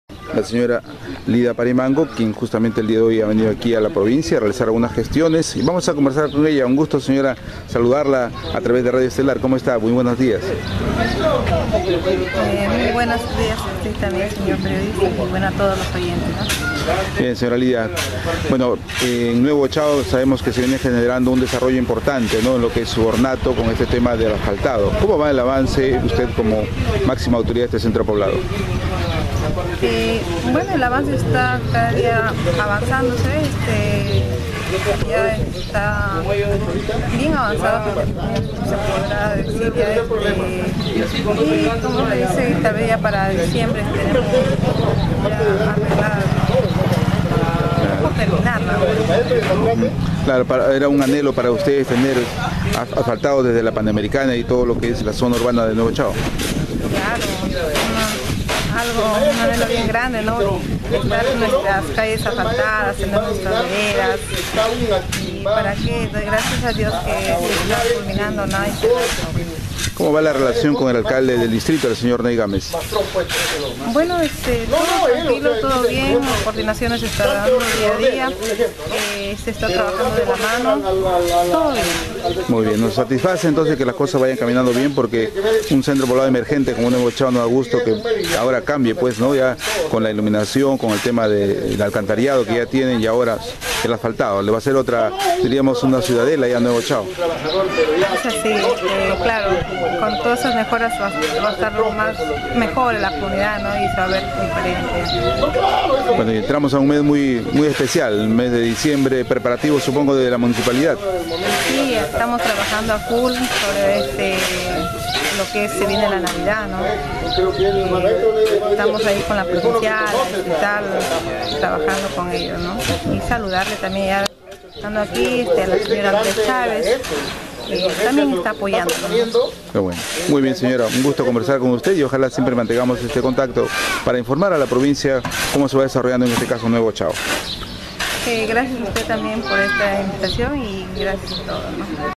Alcaldesa de Nuevo Chao
Lidia Parimango, alcaldesa del Centro poblado de Nuevo Chao, brinda declaraciones relacionada a la labor que viene desempeñando en ese importante sector poblacional.